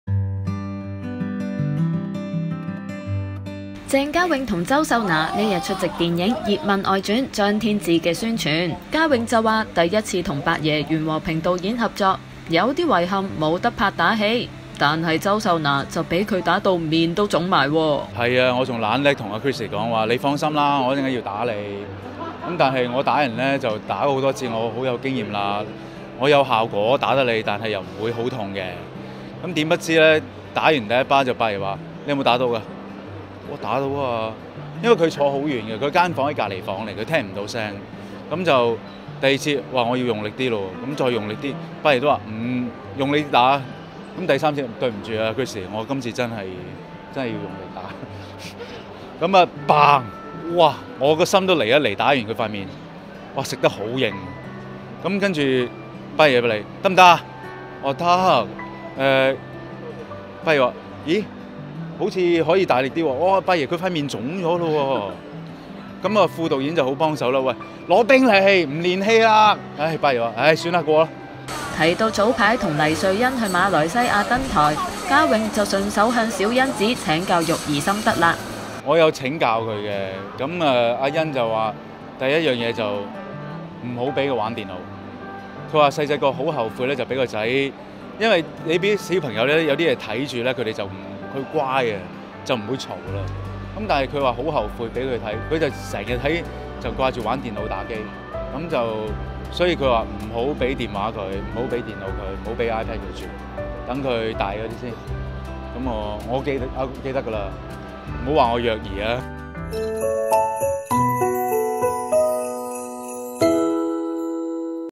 【娛樂】棘手摧花 鄭嘉穎打到周秀娜面腫 2018-12-16
鄭嘉穎日前出席電影演員見面會，首次和影壇的「八爺」袁和平導演合作的他表示略帶遺憾：「大家都知道八爺的動作戲厲害，但我這次的角色是體弱多病的，沒甚麼動作戲。」。嘉穎這次演患氣喘的黑幫少爺，而周秀娜是欠他錢的酒吧女郎，二人第一天開拍，Chrissie已被嘉穎連摑三巴，令嘉穎大感不好意思：「我還跟她說摑巴掌我很熟練，不會痛的，結果打了兩次，八爺都不過，最好只好真打了，我看到她臉都腫了。」

bmgproductionmusic